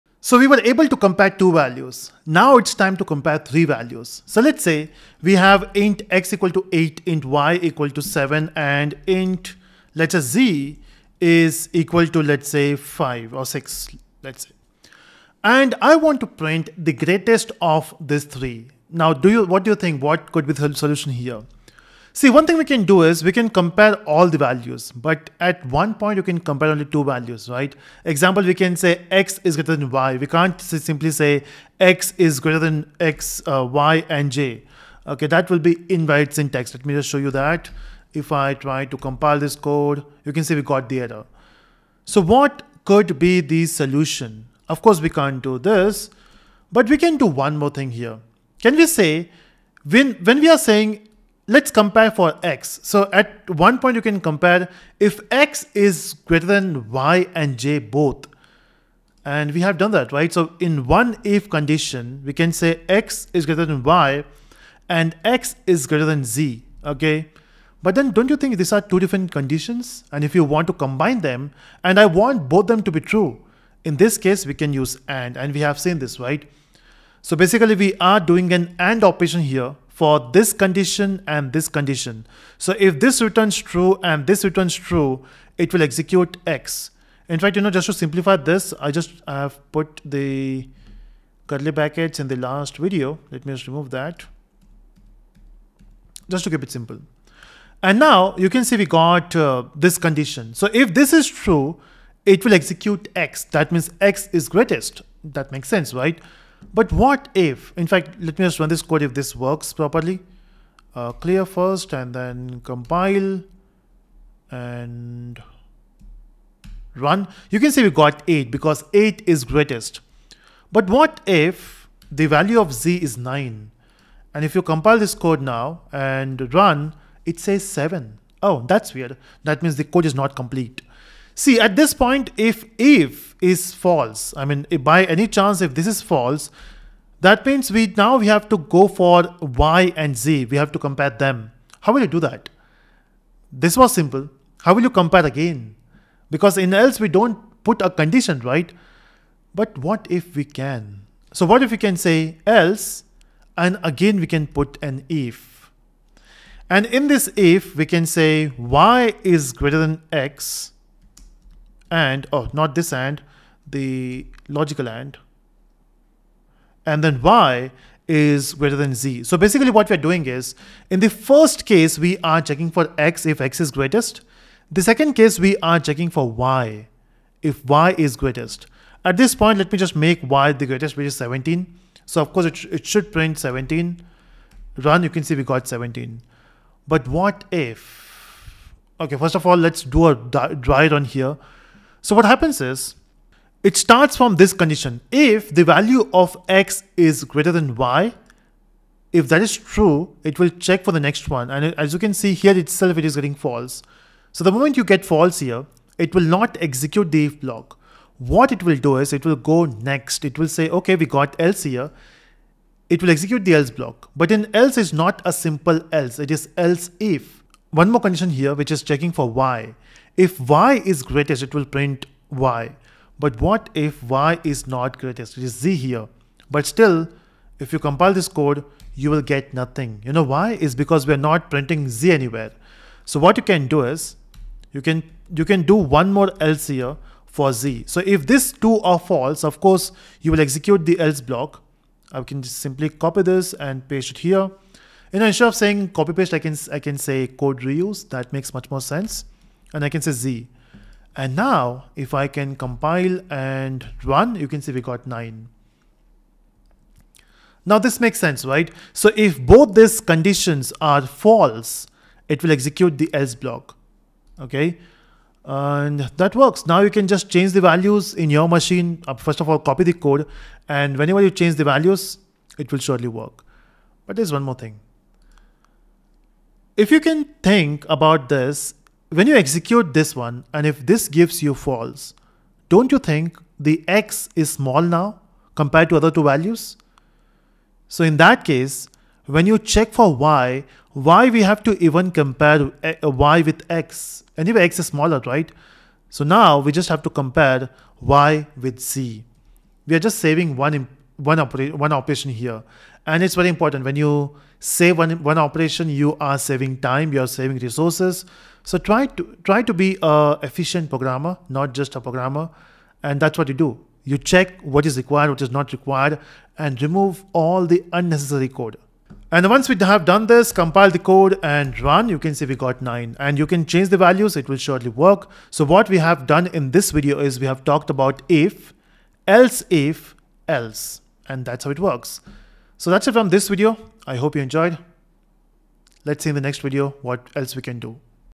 #13 If Else If in Java
In this lecture we are discussing:
1) what is if-else-if ladder?
2) syntax of if-else-if ladder
3) example of if-else-if

-- if-else-if ladder is a series of if, else if or else statements that are executed based on the boolean condition.
-- if else if ladder is used to test multiple conditions

syntax of if-else-if ladder

if(condition1)
    //block of code
else if(condition2){
    //block of code
else if(condition3)
    //block of code
else{
    //block of code

Note: only one block is executed from all. If all condition is false then else part executed

-- check greatest number from three number.
      int x=8;
      int y=7;
      int z=6;
        if(x y && x z)
        System.out.println("x is greater");
        else if(y z)
        System.out.println("y is greater");
        else
        System.out.println("z is greater");